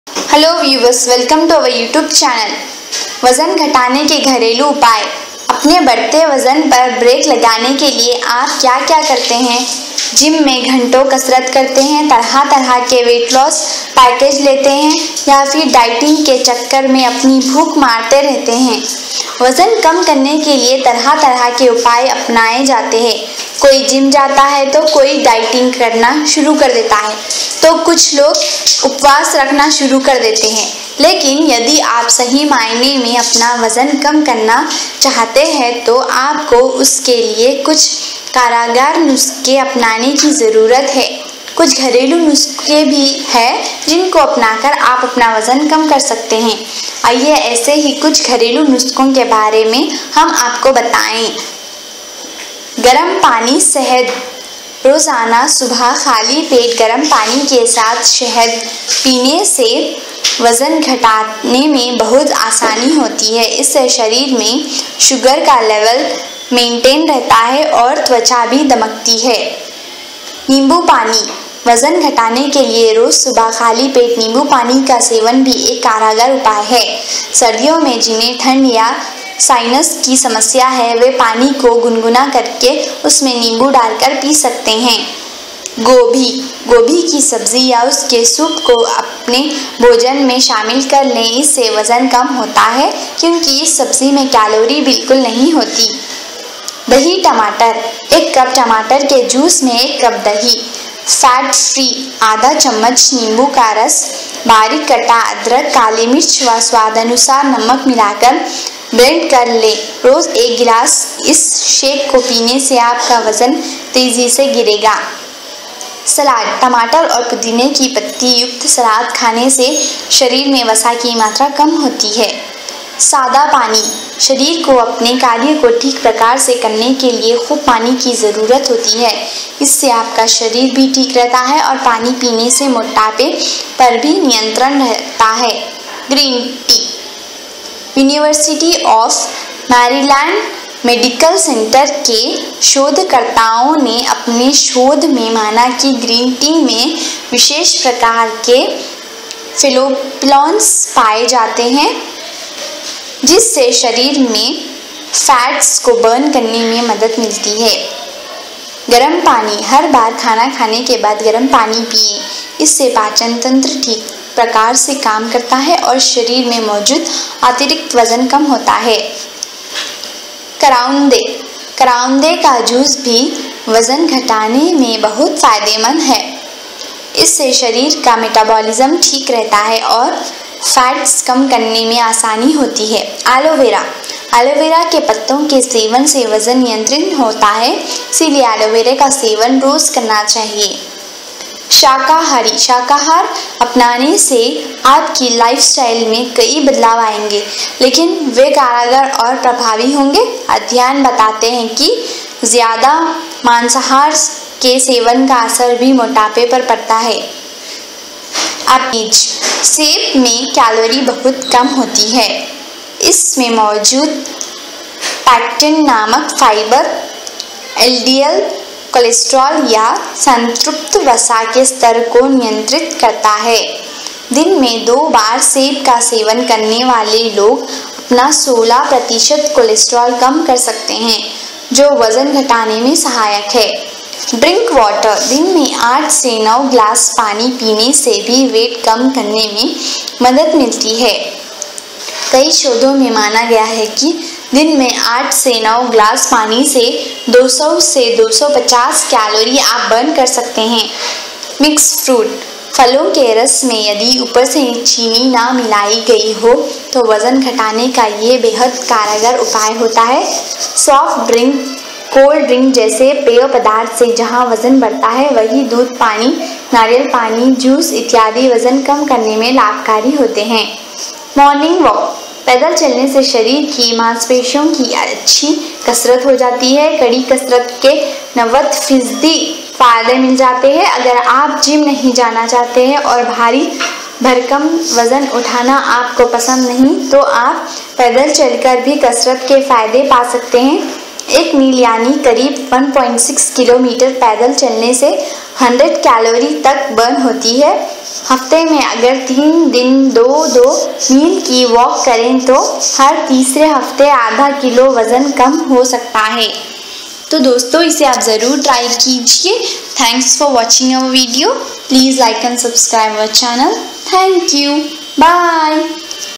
१५ सबसे प्रभावी वज़न घटाने के घरेलू उपाय | Most Effective Home Remedy for Weight Loss
15 सबसे प्रभावी वज़न घटाने के घरेलू उपाय | Most Effective Home Remedy for Weight Loss

1. गर्म पानी के साथ शहद से वजन कम करे
2. नींबू पानी से वजन कम करे
3. गोभी से वजन कम करे
4. टमाटर के जूस और दही से वजन कम करे
5. Normal Water for weight Loss
6. गरम पानी से वजन कम करे
7. शाकाहार से वजन कम करे
8. करौंदे से वजन कम करे
9. Salad से वजन कम करे
10. Aloveera से वजन कम करे
11. Green tea for weight Loss
12. Apple for weight Loss
13. Drink Water for quick weight Loss
14. Mix Fruit for quick weight Loss
15. Morning Walk for quick weight Loss

15 दिन में मोटापा और पेट की चर्बी घटायें, instant Weight loss Diet plan, How to get flat tummy